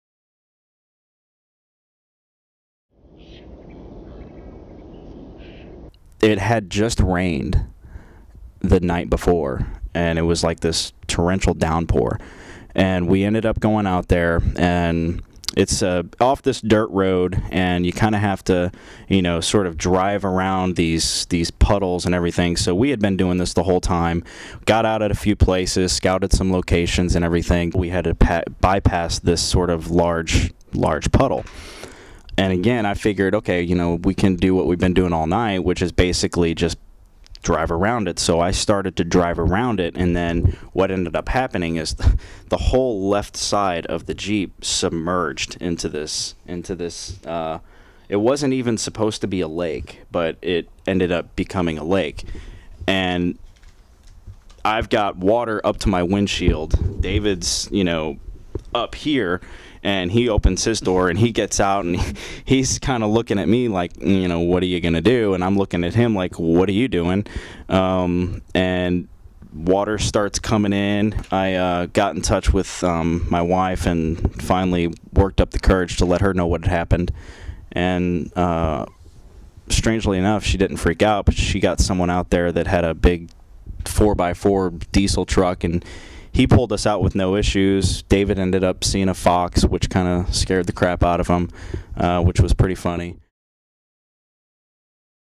Tomorrow Mourning: The Flooded Car
Jeff & David discuss a story from Tomorrow Mourning shooting.